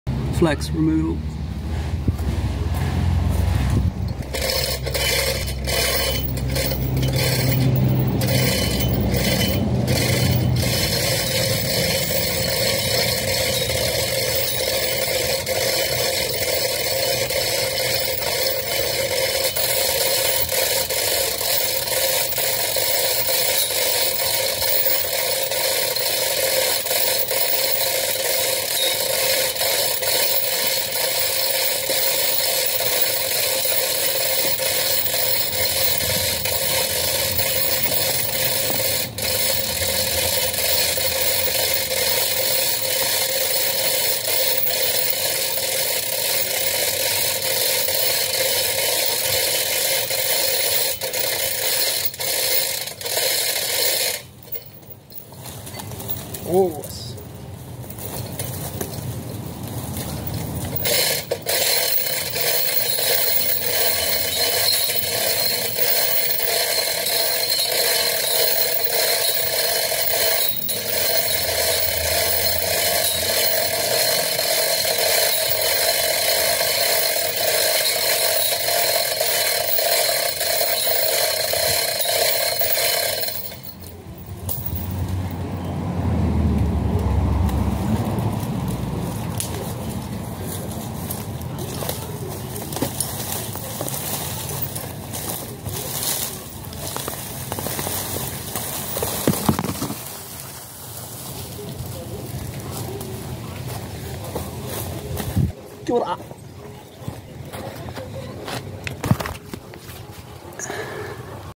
Flax (Harakeke) Removal - Momentous Services - Ratonga hira
Method allows Harakeke to be replanted just as the one shown was.